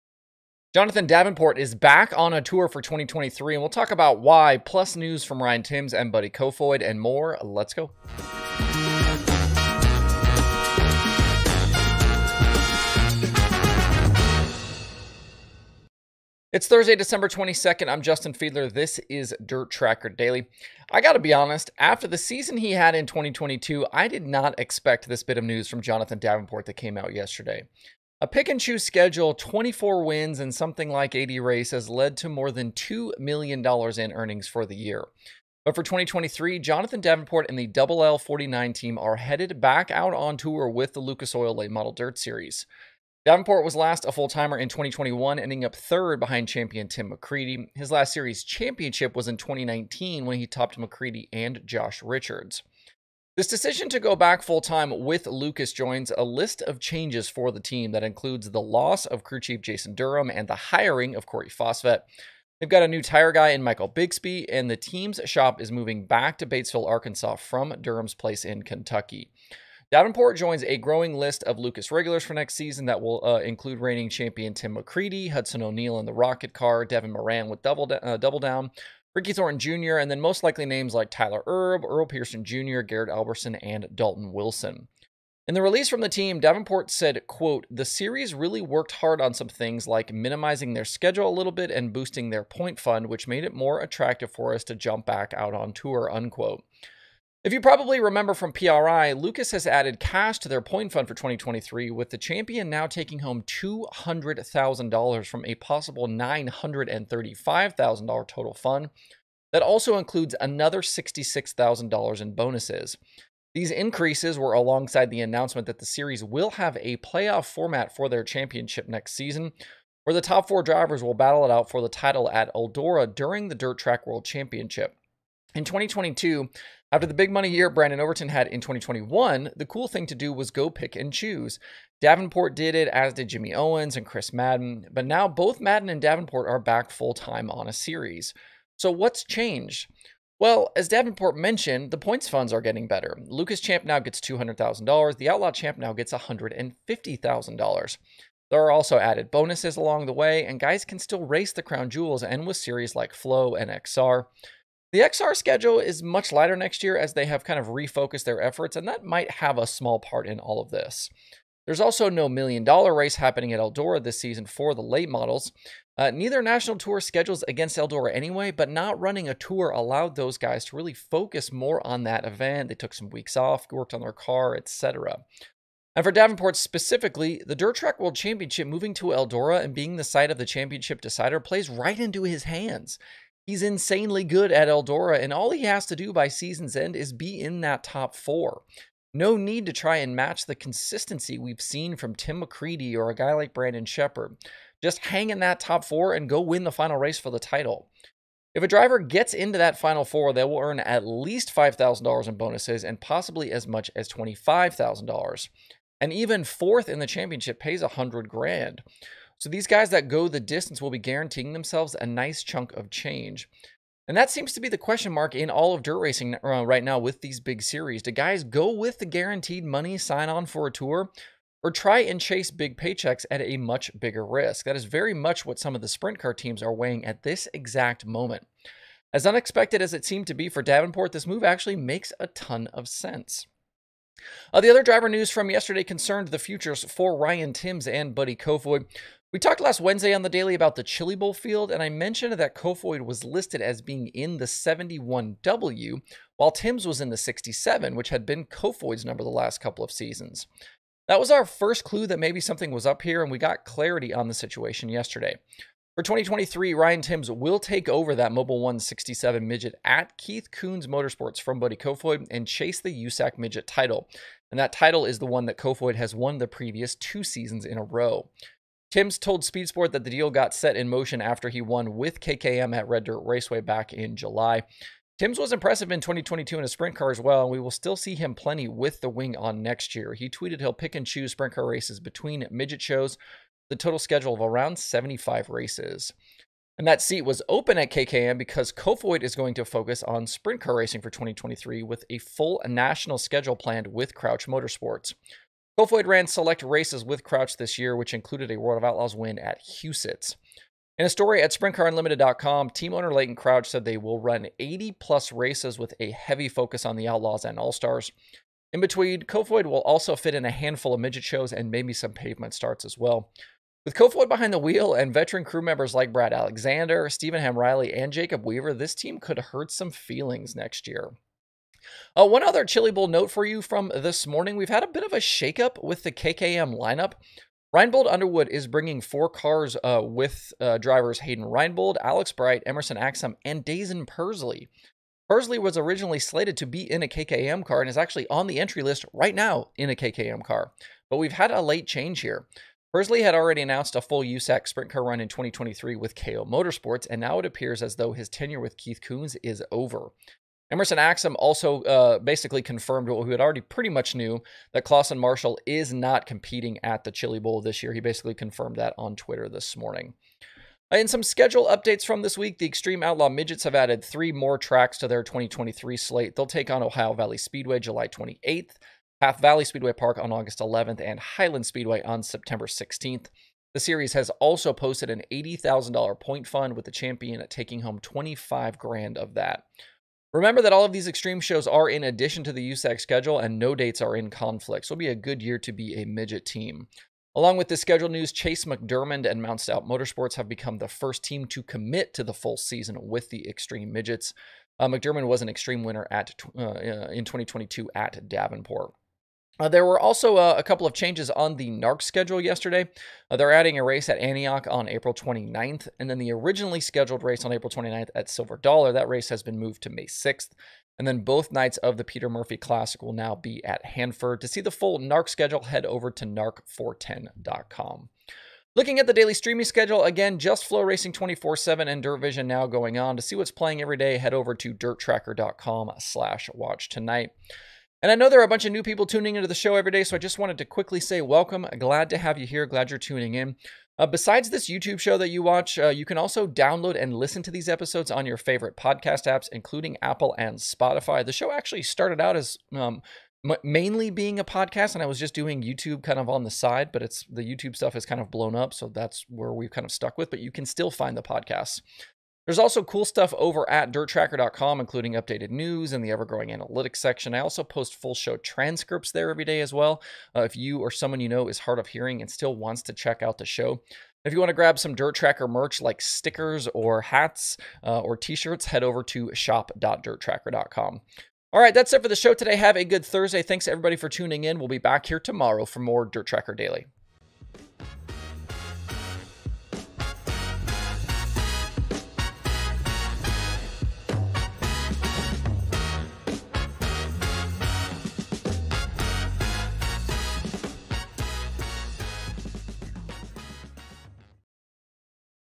The reasons behind Jonathan Davenport's return to Lucas full time
Like my little LED board?

Protect your hearing at the race track!

Superman is back on tour with the Lucas Oil Late Model Dirt Series, and we'll talk about why it make so much sense. We've also got news from Ryan Timms, Buddy Kofoid, and more.